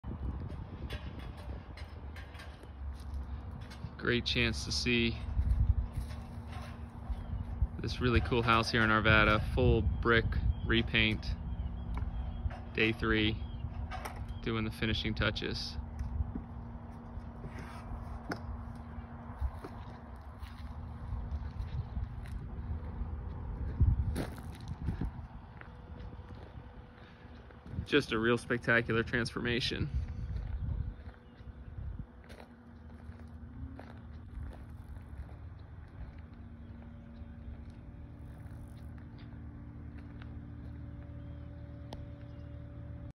Day 3! House Painting Arvada, CO.
Day 3. House painters Arvada, CO.  Exterior residential home painters. Brick house painting.